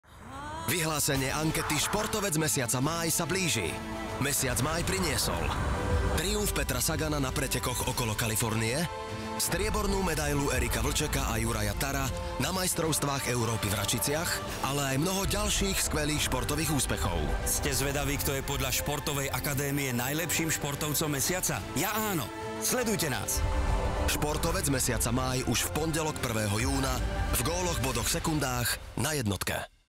Športovec mesiaca: Upútavka v RTVS, máj 2015
Športovec mesiaca: Upútavka v RTVS, máj 2015, Marcel Merčiak
Spoluorganizátor tradičnej ankety Športovec roka spoločnosť B&D, s.r.o. v spolupráci s RTVS a LOCO Agency vyhlasujú novú anketu „Športovec mesiaca“. Prvé hlasovanie športových redakcií vybraných slovenských médií sa konalo koncom marca 2015, keď sa vyberal Športovec mesiaca za aktuálny mesiac. Výsledky exkluzívne zverejňuje RTVS - teraz v pondelok 1. júna 2015, potom sa publikujú aj na webe a v ostatných partnerských médiách.